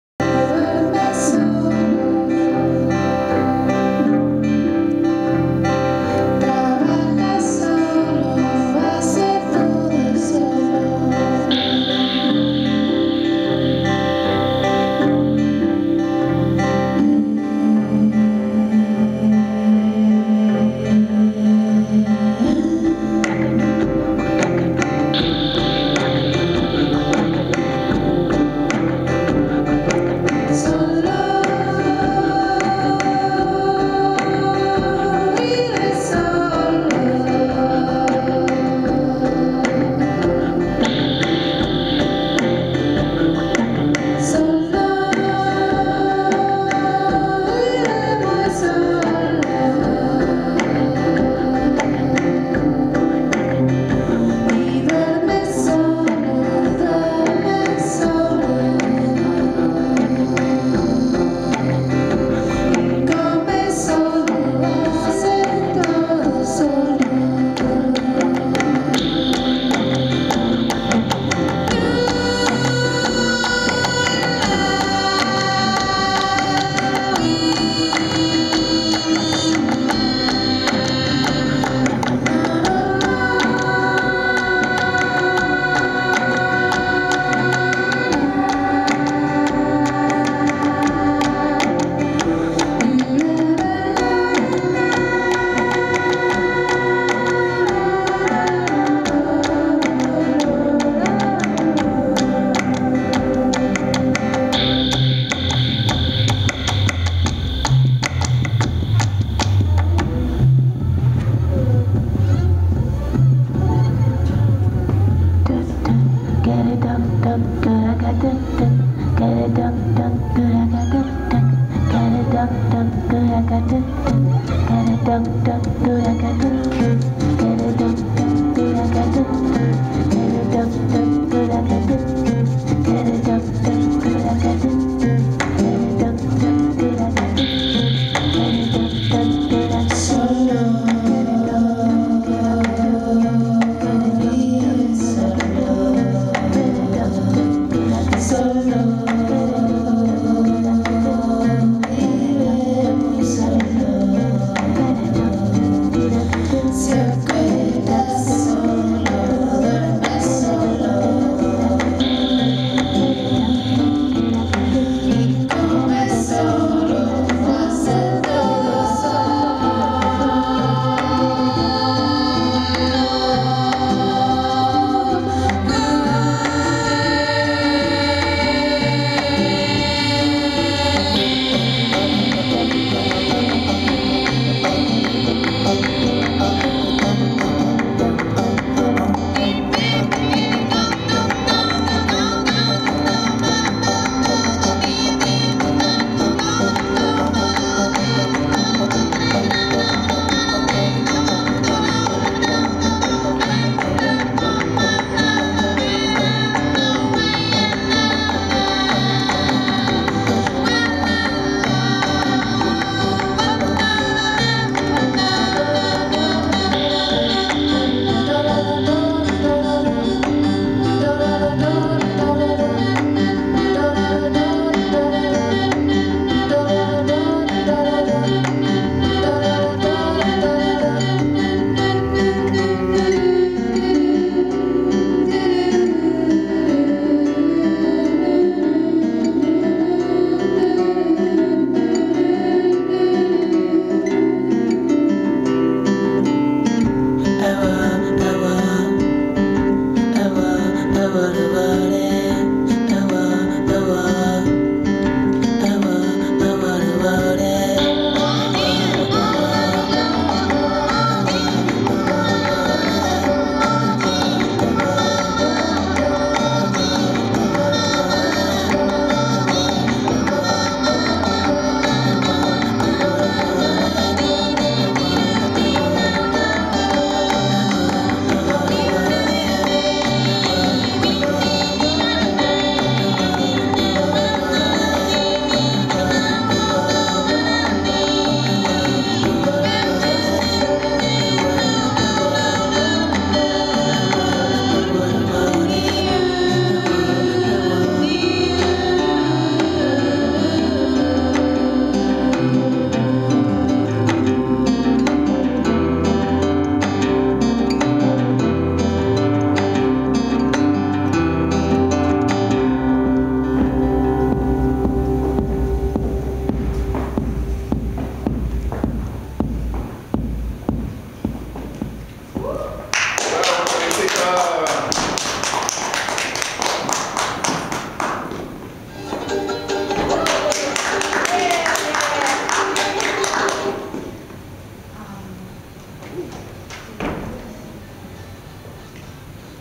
Improv Solo 12-2010
Solo performed by Callie Chapman as part of the fundraiser for Chile, Afghanistan and Haiti relief.  At Mass Art, December 9, 2010